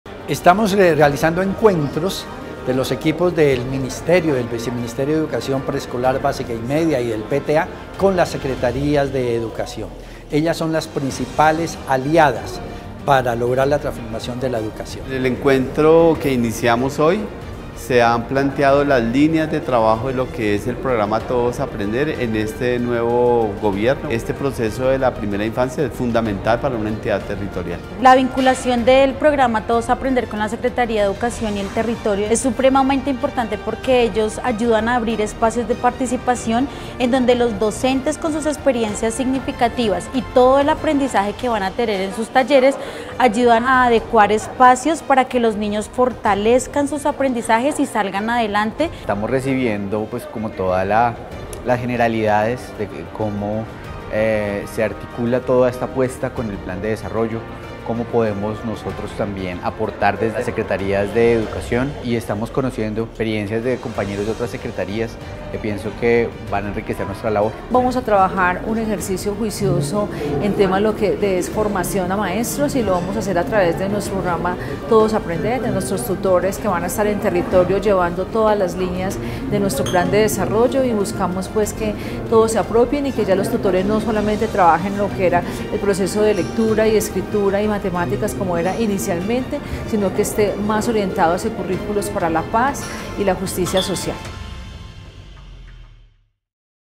Encuentro Regional del PTA con las Secretarías de Educación Certificadas
¡El PTA se fortalece y sigue avanzando para brindar acompañamiento situado y formación continua! Con un diálogo de saberes se vivió el I Encuentro Regional del PTA con las Secretarías de Educación Certificadas.